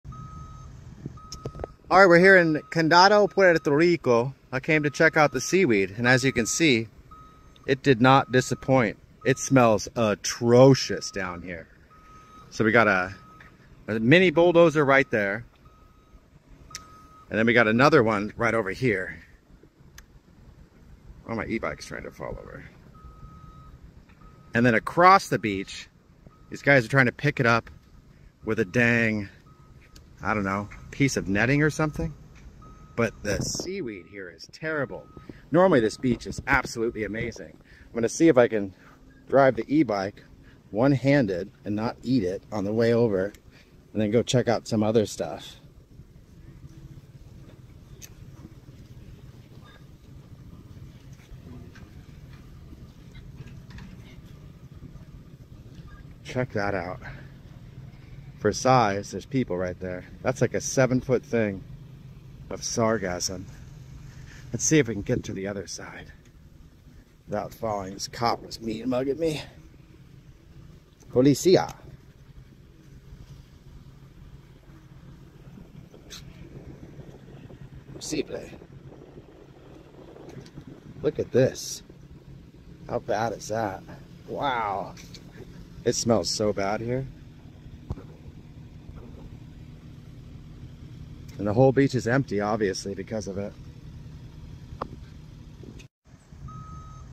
seaweed is taking over Puerto Rico’s beaches
argassum seaweed is taking over Puerto Rico’s beaches — and it’s changing the island’s coastline, tourism, and marine life. From San Juan to Vieques and Culebra, massive waves of brown seaweed are washing ashore, creating thick mats along the sand, releasing a strong odor, and threatening coral reefs. This video covers what’s causing the sargassum bloom, how climate change and ocean currents are fueling it, and what visitors and locals need to know before hitting the beach. 🏝️🐠

Whether you’re planning a Puerto Rico vacation, live on the island, or just want to understand the Caribbean seaweed problem, we break down the science, environmental impact, and real-world effects of this growing crisis. 🌎♻️

💬 Share your experiences with sargassum in the comments — have you seen it where you live or traveled?